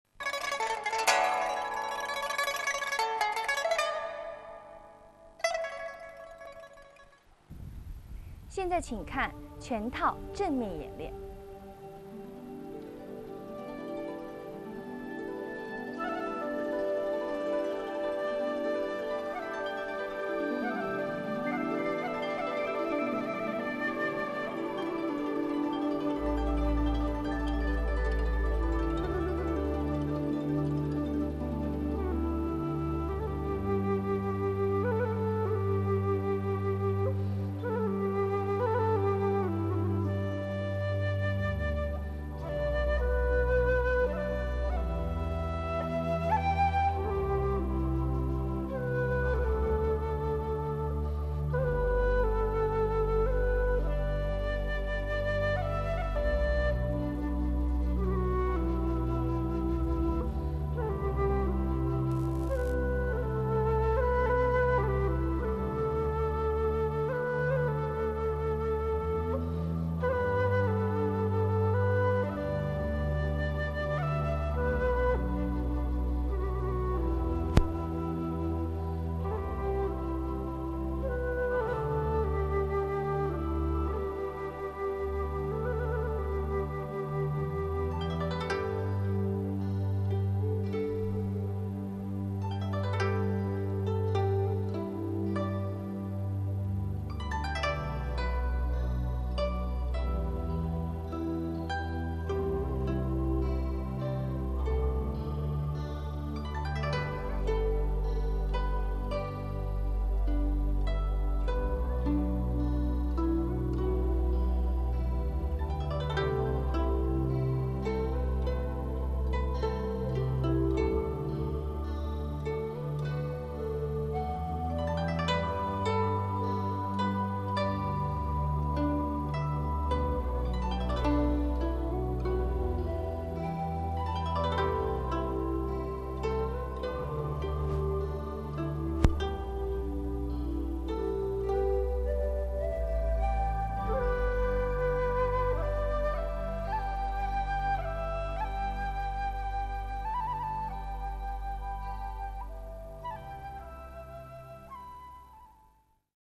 Wu-Style 9 Form Tai Chi Chuan
Original Tai Chi instructional VCD video by Sifu Amin Wu (Mandarin Chinese version) available on website.